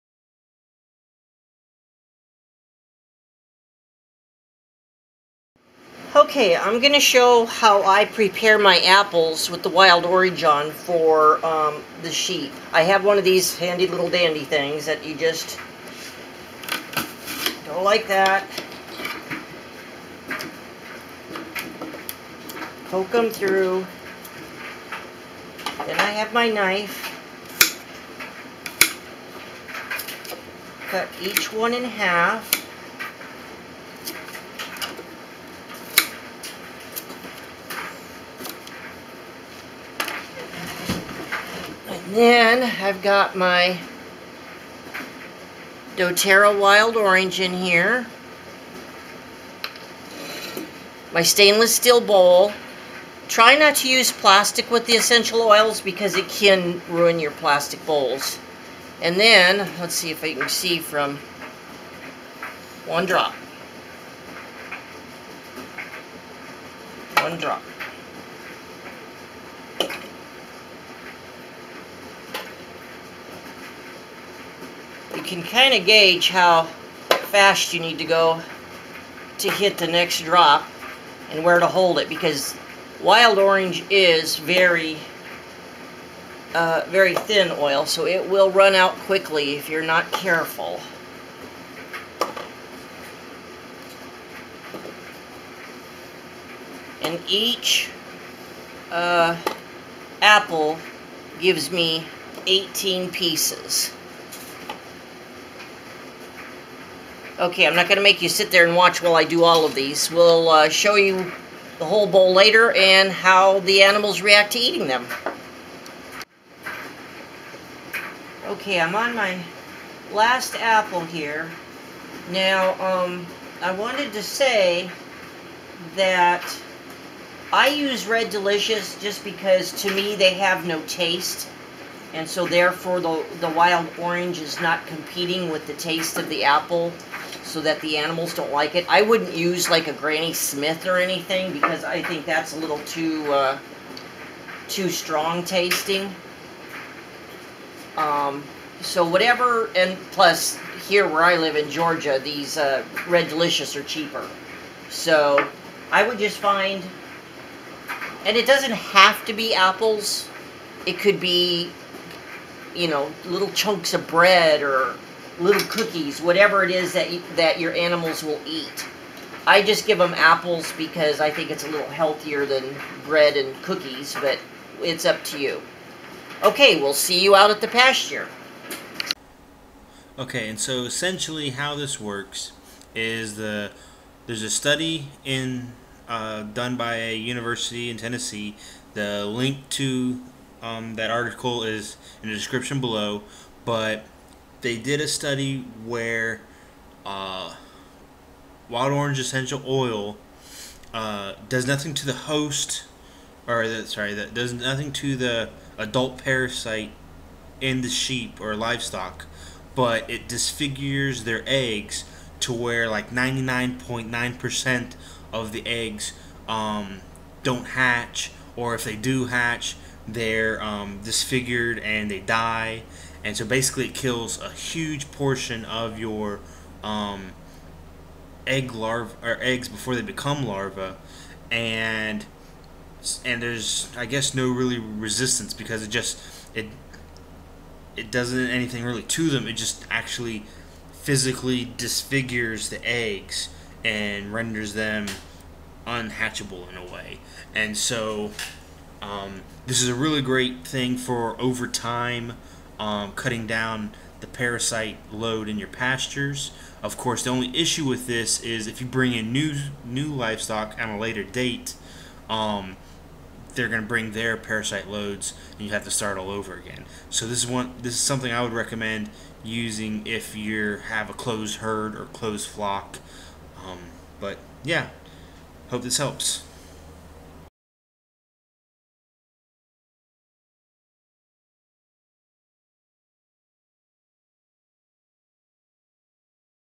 Destroying Parasite Eggs with Essential Oils.
Title says it all! How a simple essential oil can destroy or render unhatchable various parasitic eggs.